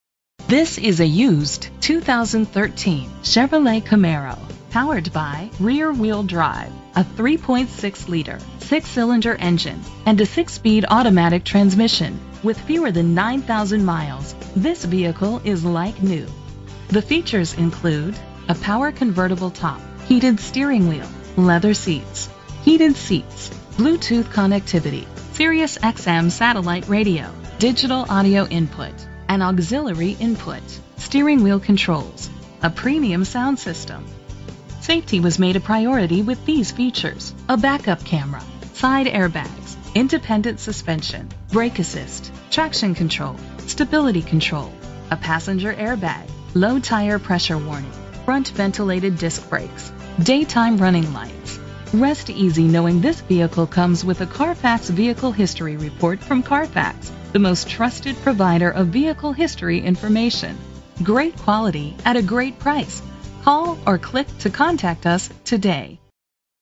2013 Chevrolet Camaro - Perrysburg OH
This vehicle represents similar vehicles available at Ed Schmidt Auto Group.
Year: 2013
Make: Chevrolet
Model: Camaro
Trim: 2LT
Engine: 3.6 liter 6 cylinder 24 valve GDI DOHC
Transmission:
Color: Rally Yellow
Mileage: 8517
Address:
26875 N Dixie Hwy